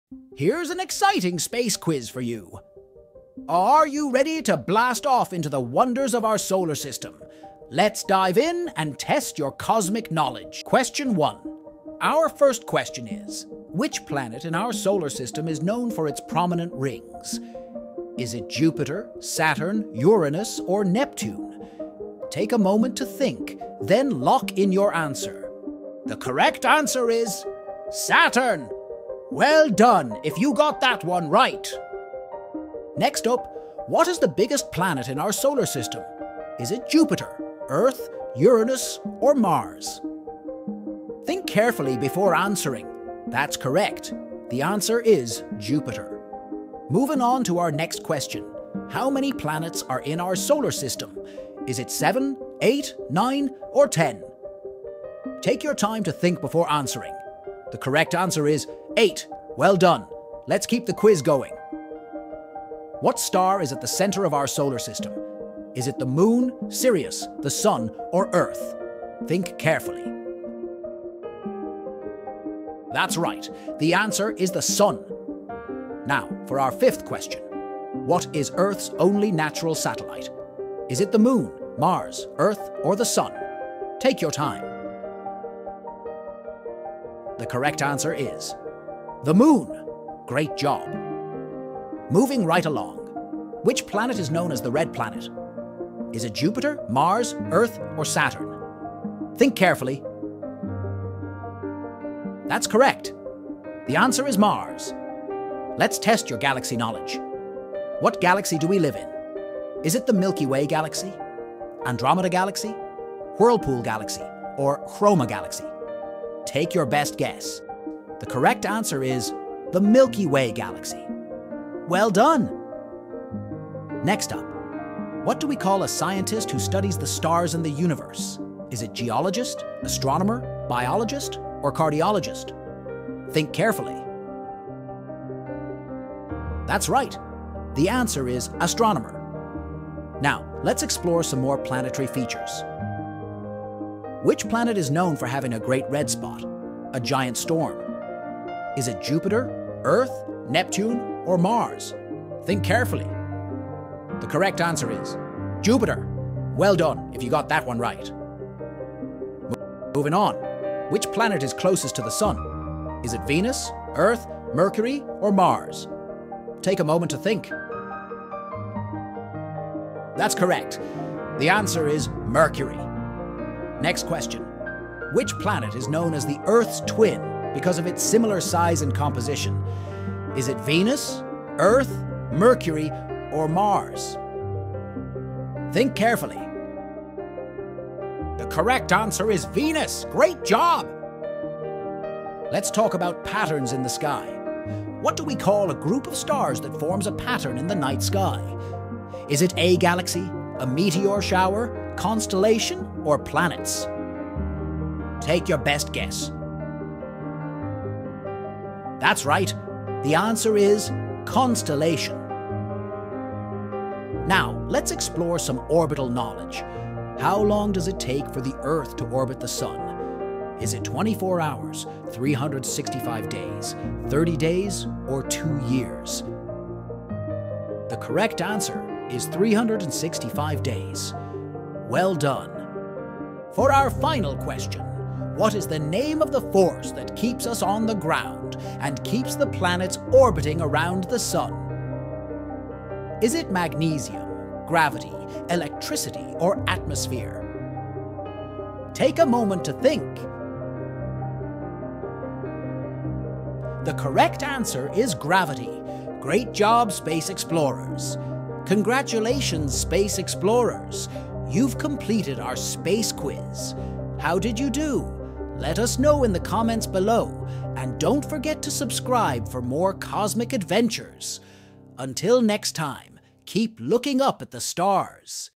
🚀 Fun Space Quiz for Kids! Test Your Solar System Knowledge!
Welcome to our exciting Space Quiz for Kids! 🌌 Join us as we journey through the wonders of our solar system and explore fascinating facts about planets, stars, and more! 🪐🌟 Get ready to challenge your knowledge and have a blast with each question. From identifying planets to understanding cosmic phenomena, this quiz is perfect for young space enthusiasts! Let's dive in and see how much you know about the mysteries of space! 🛸